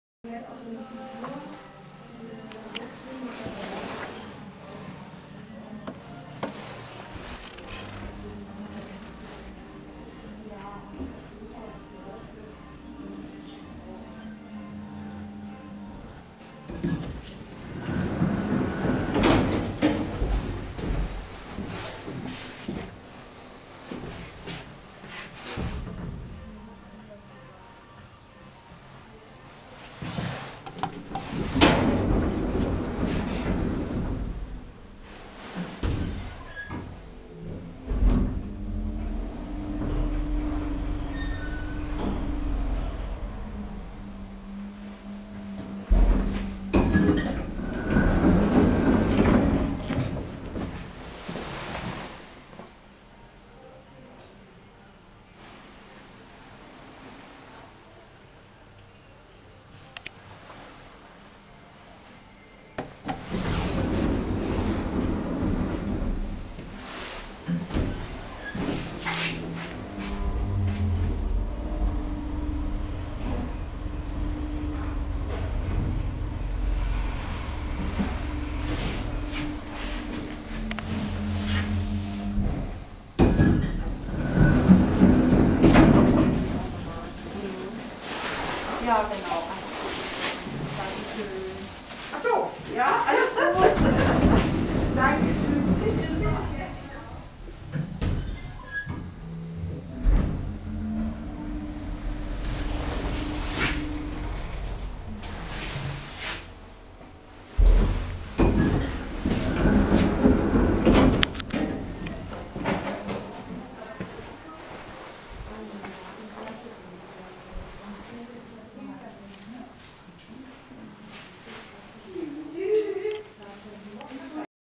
Hameln, Modehaus Kolle
Hersteller / Brand: Lödige
Baujahr / Year of Construction: 1997
Tragfähigkeit / Capacity: 13 Pers.
Etagen / Floors: 5 (K, E1, E2, 1, 2)

E2 liegt drei Treppenstufen höher als E1. Die zweite Etage ist nicht öffentlich zugänglich.
Dieser Aufzug ersetzt eine Anlage aus den 50ern oder 60ern; diese hatte rechts noch eine dritte Tür, die nur im zweiten Obergeschoss geöffnet werden konnte.

E2 (= Ground floor 2) is three stair steps higher than E1 (= Ground floor 1). The second floor is not open for public.
This elevator replaces an older one from the 50s or 60s that has had a third door on the right side which could be opened only on second floor.